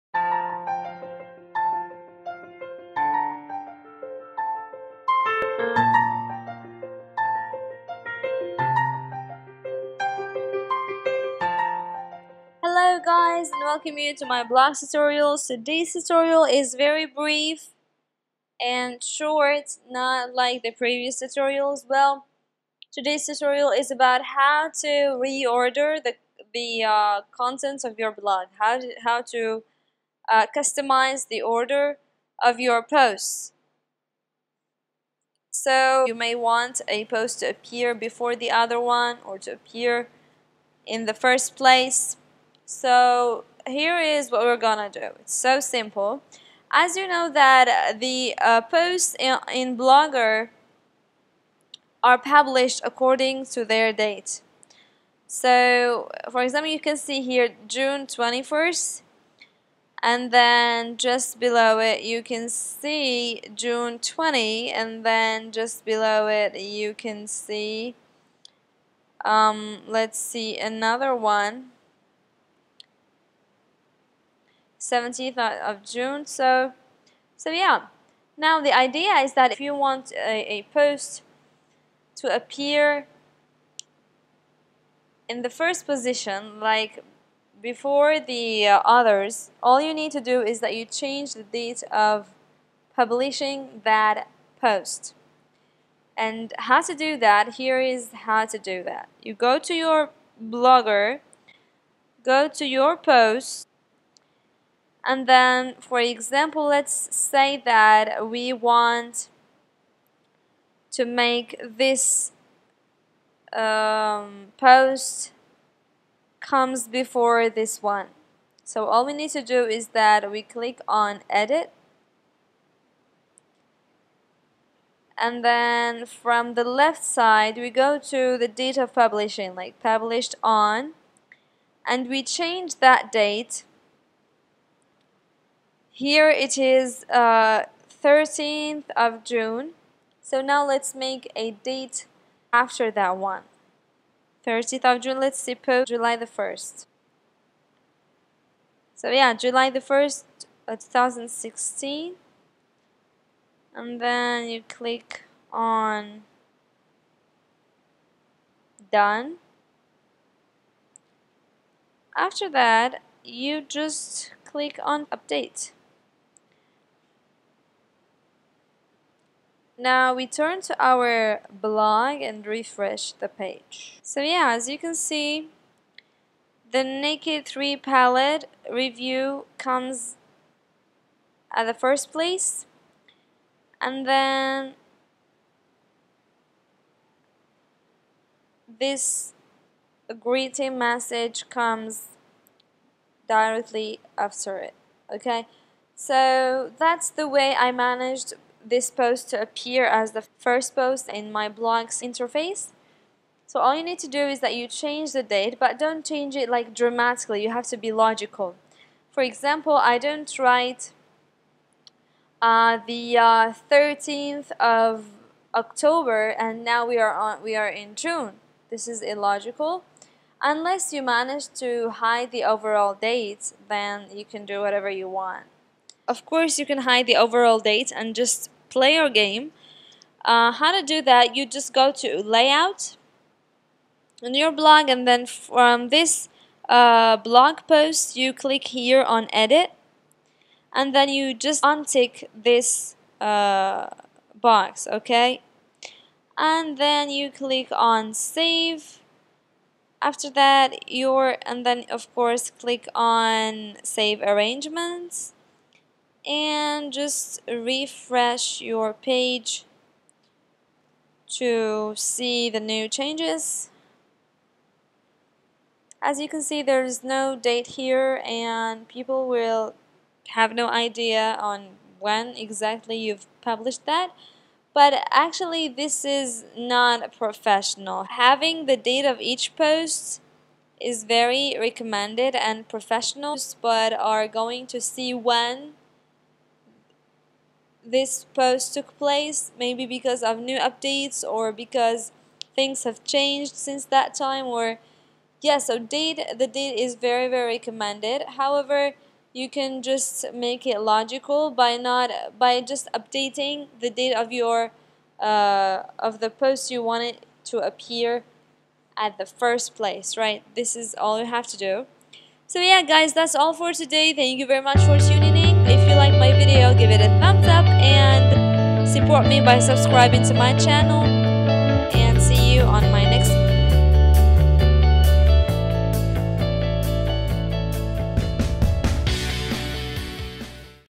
How to reorder (rearrange) your blog's posts in the way you want !
Okay !. now you know another trick to make your blog more customised ! .